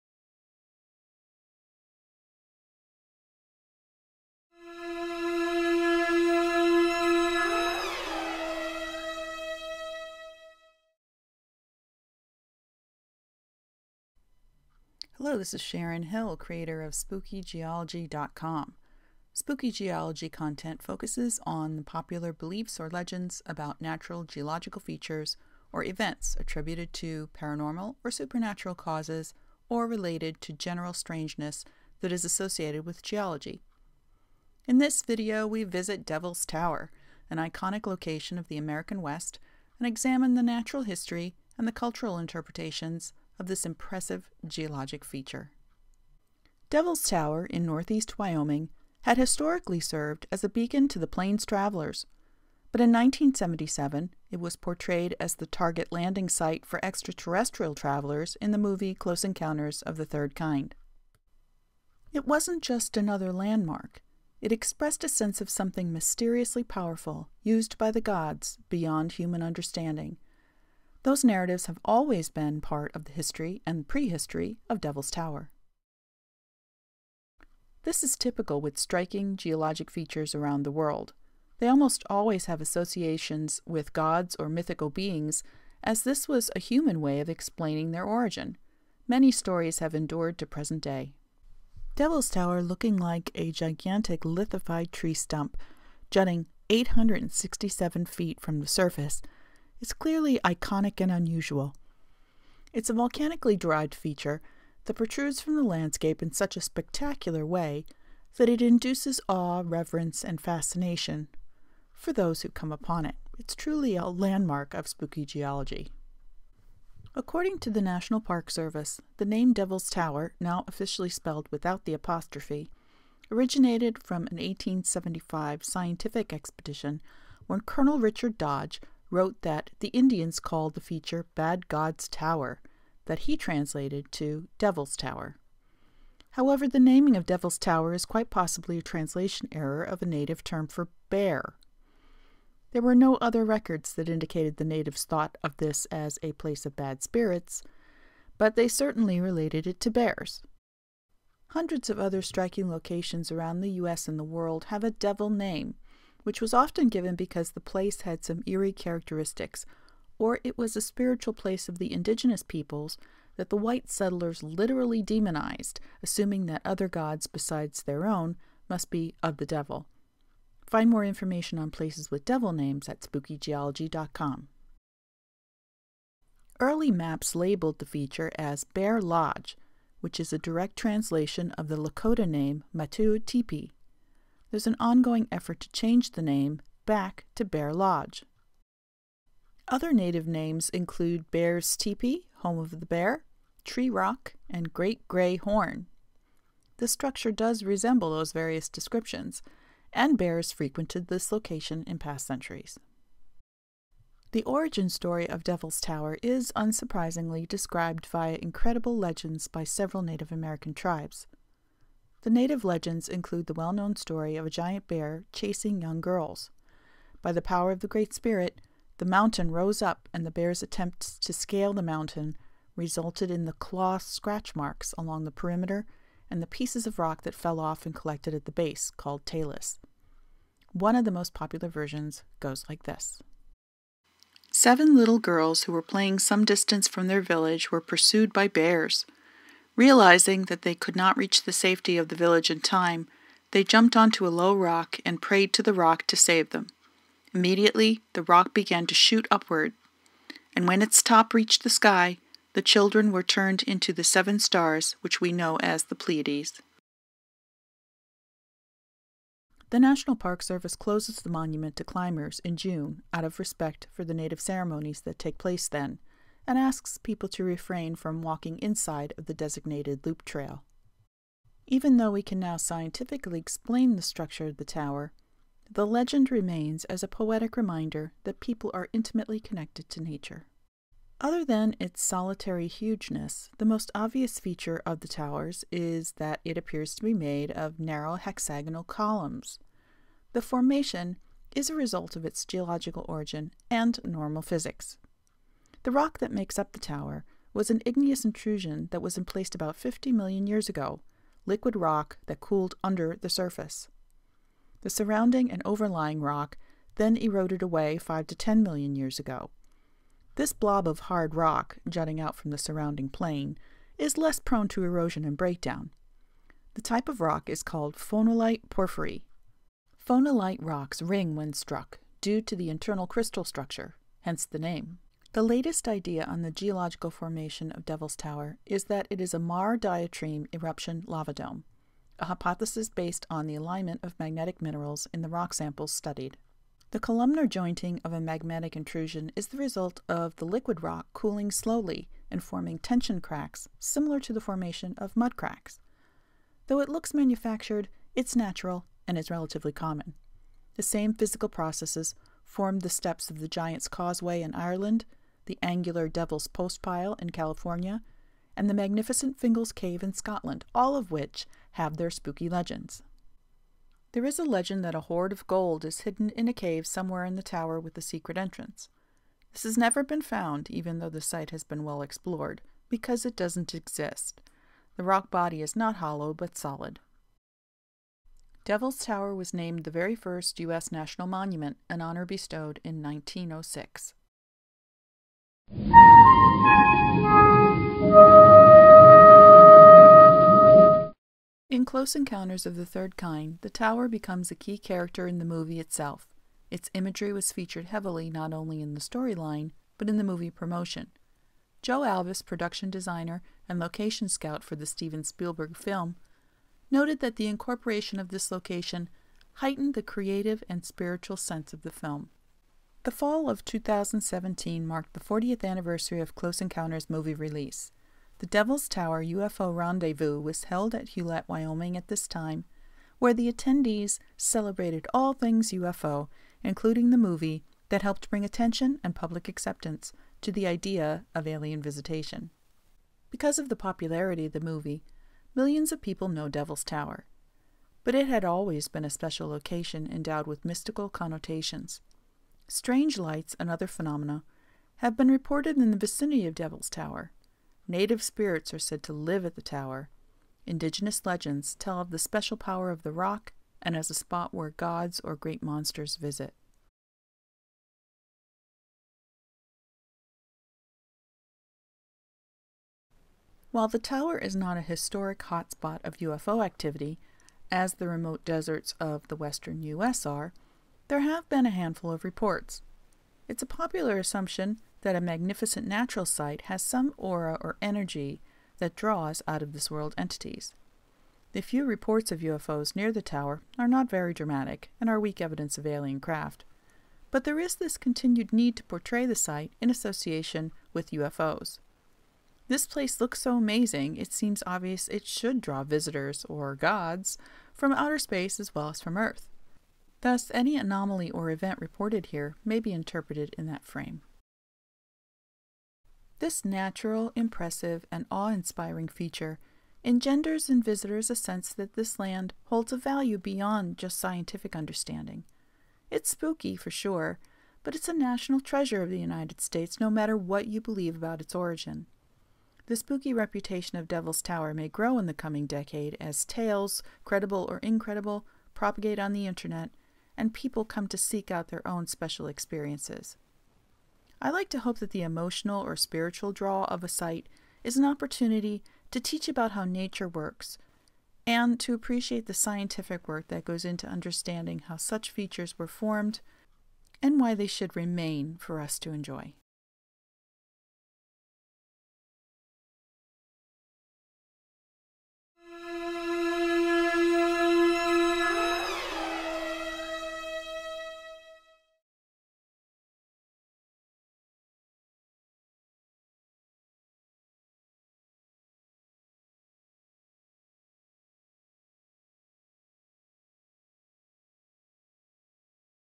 Devils Tower: Science and Myth
Spooky Geology presents the mystery, myth, legend, and geology about the Devils Tower National Monument including its infamous and continuing association with UFOs.